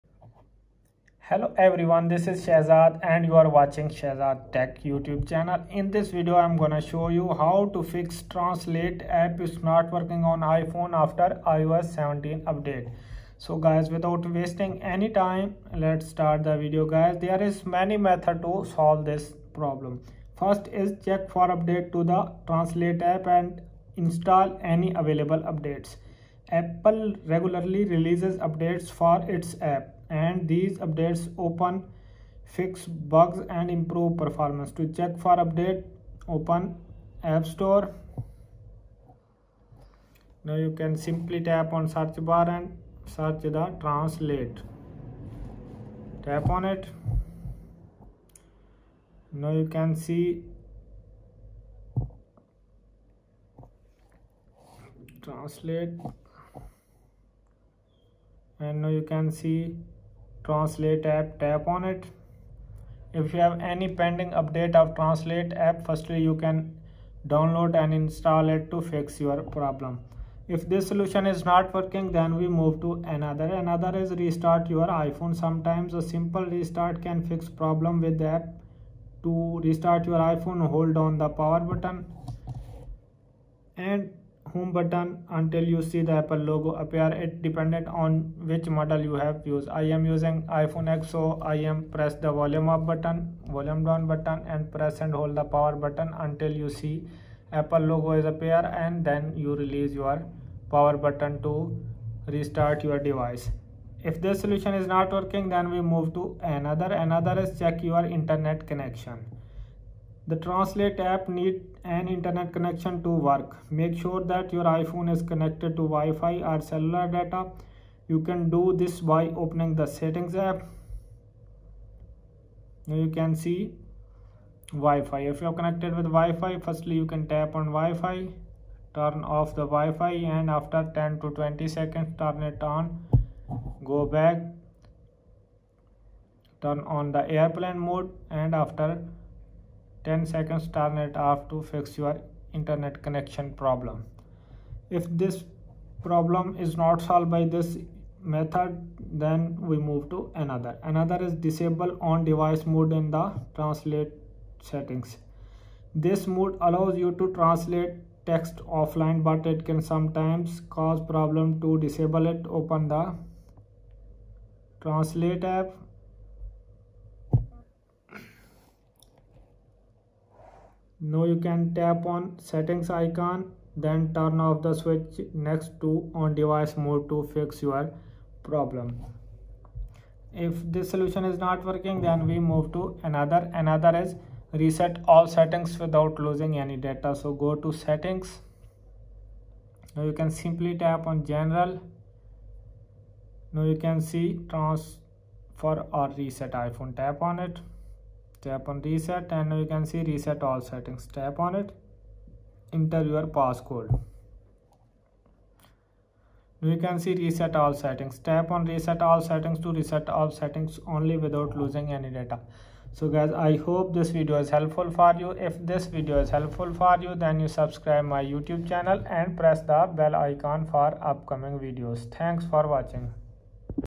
How To Fix Translate App Not Working On iPhone After iOS 17 Update Fixed 💯
After Watching This Video We Learn How To Fix Translate App Not Working On iPhone After iOS 17 Update Fixed 💯